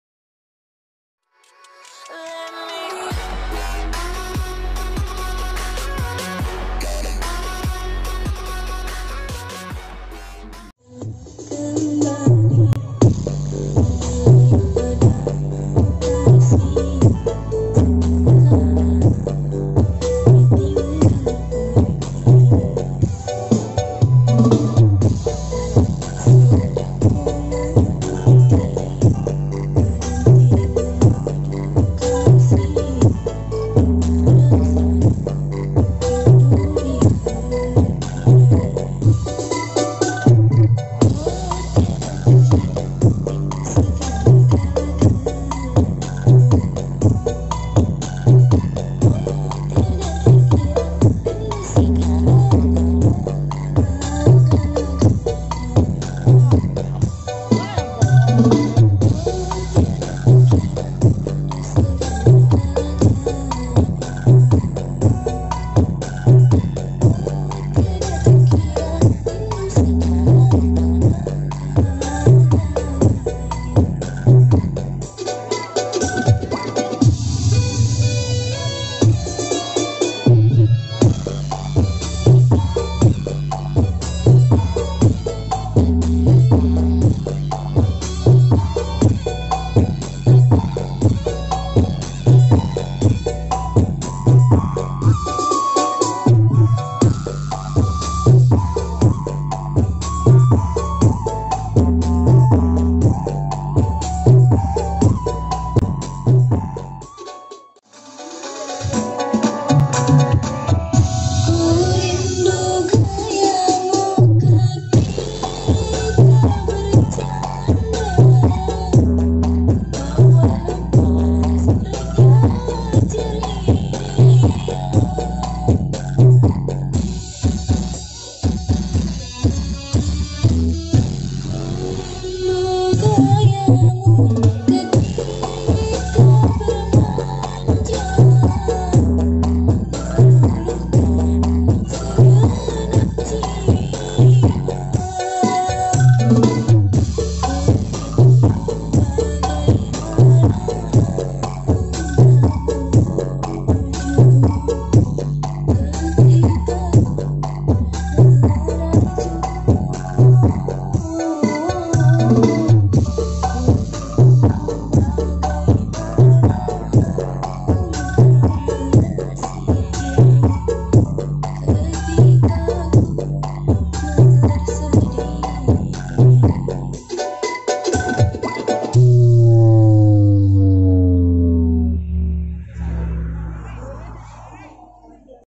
CEK SOUND GIANT 2 DI BORO JABUNG MALANG
Seperti Biasanya,Semakin malam pengunjung semakin banyak Yang datang !!